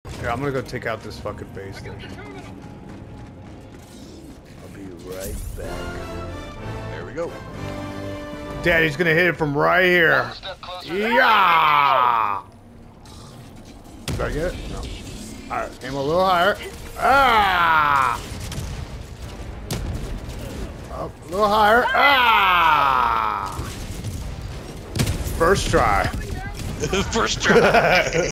First Try (trust me)
I effortlessly take out a fabricator